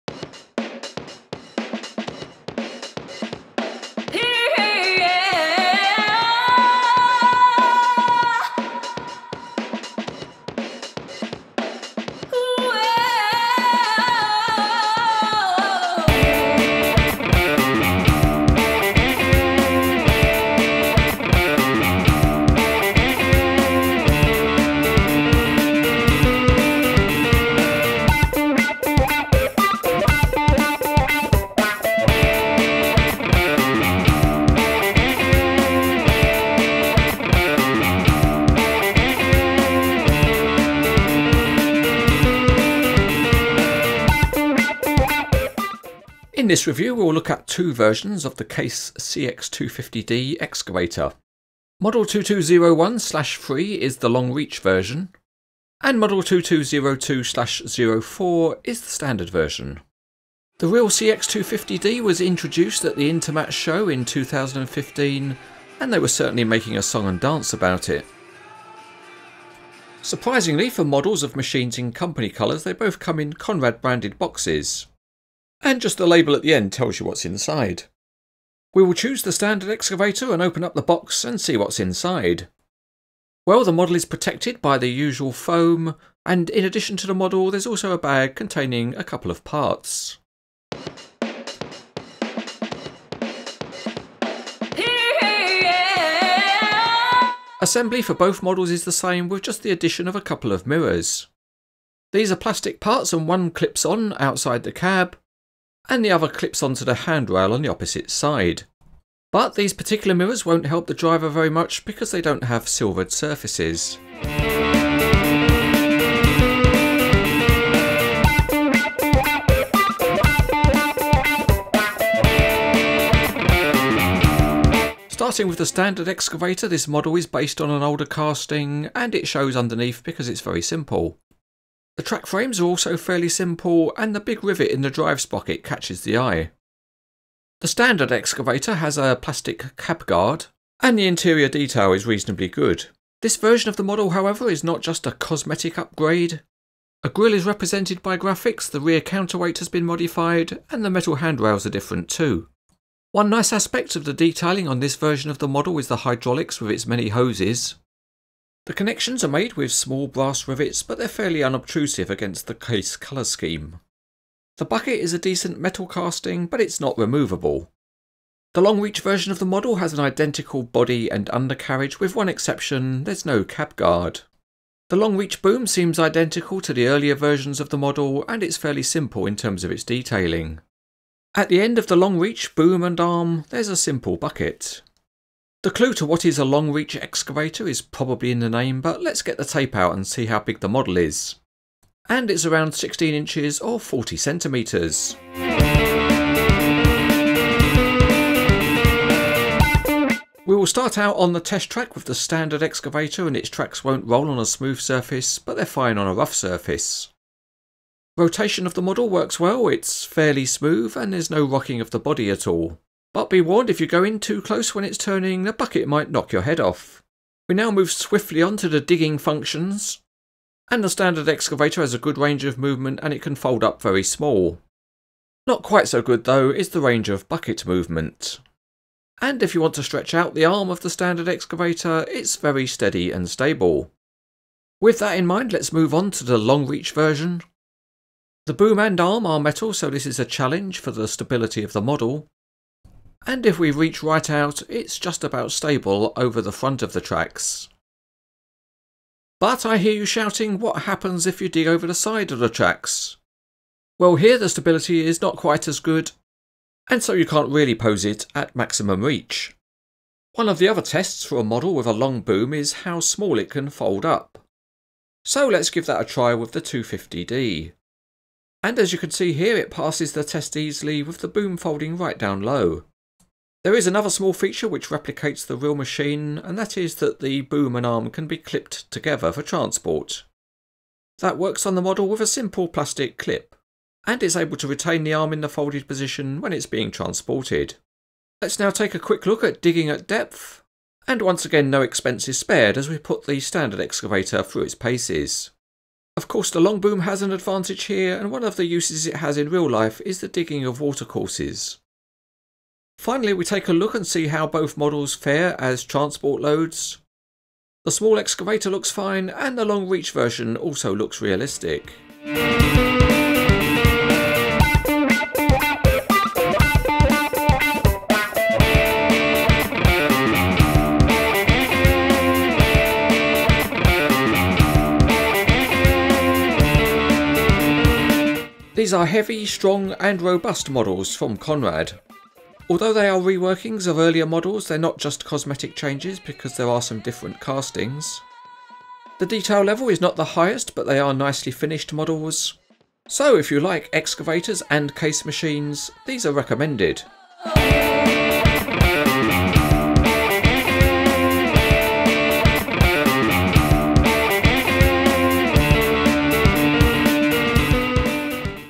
Conrad Case CX250D Excavators by Cranes Etc TV
This is the Cranes Etc TV review of Conrad's 1/50 scale models of the Case CX250D in standard and long reach configuration.  The model numbers are 2201/03 and 2202/04 .  The full reviews are on the Cranes Etc